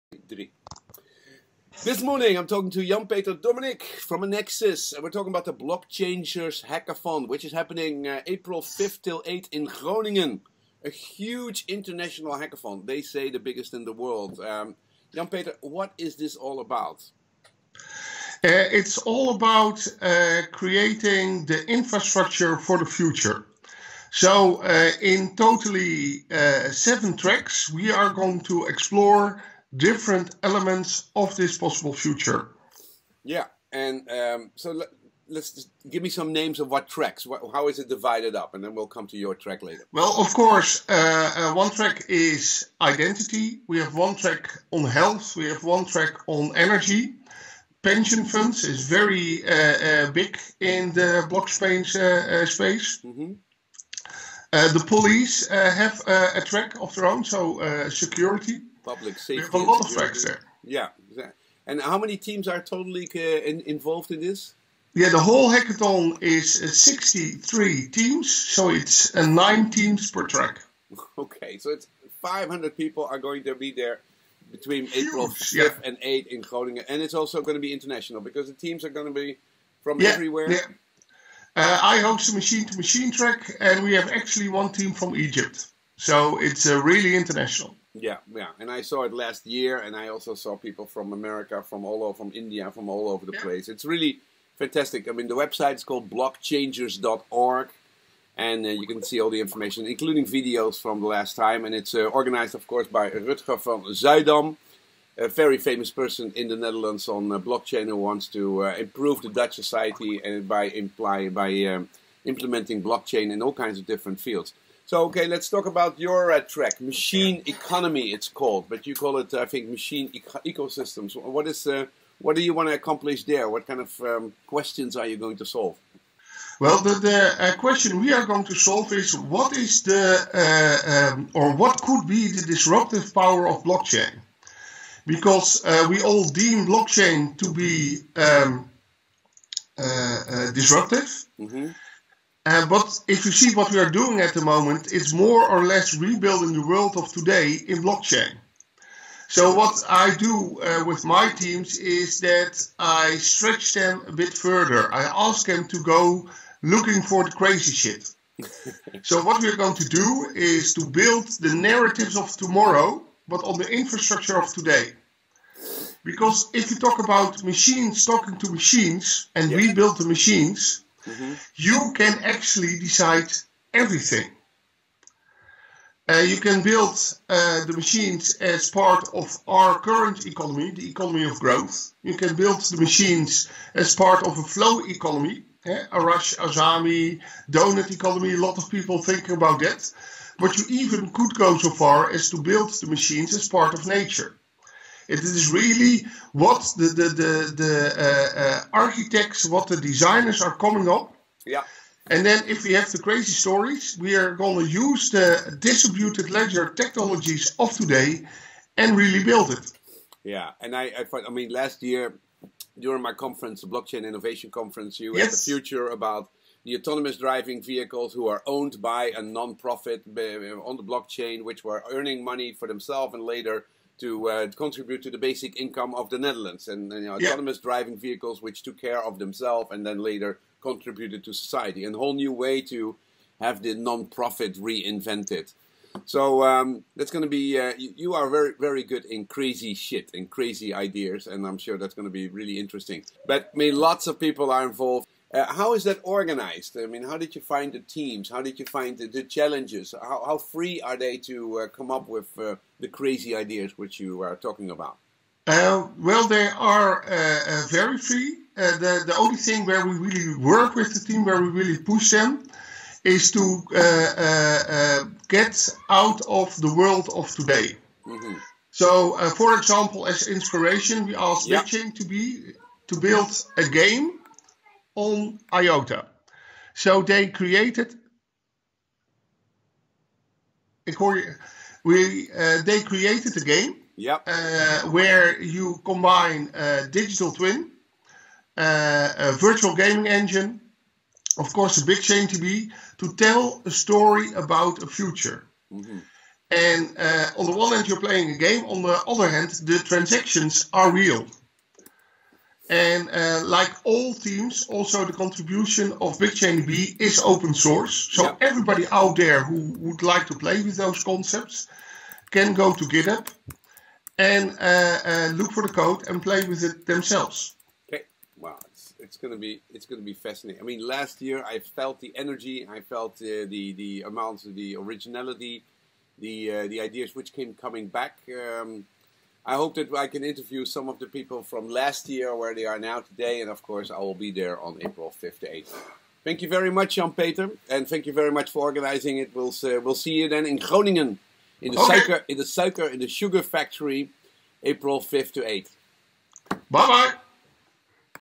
Blockchaingers Hackathon 2018: Interview Jan-Peter (Machine Economy track & Enexis)
World's largest blockchain hackathon, organized by Blockchaingers, took place in Groningen from 5-8 April. It included 63 international teams in 7 tracks: Digital Nation Infrastructure, Energy Transition, Global Digital Identity, Health, Machine Economy, Public Safety & Security and the Future of Pensions.

The visionary Jan-Peter Doomernik (Enexis), the Machine economy track host, spoke at BIC17 about autonomous cars owned by nobody, but which still can be used by everybody by means of using blockchain technology. I talk with him about the hackathon, the challenges, the teams and his expectations of the hackathon. It's all about creating the infrastructure for the future. The hackathon is organized by Rutger van Zuidam who will be a speaker at the Blockchain Innovation Conference 2018 to talk about the takeaways and results of the hackathon!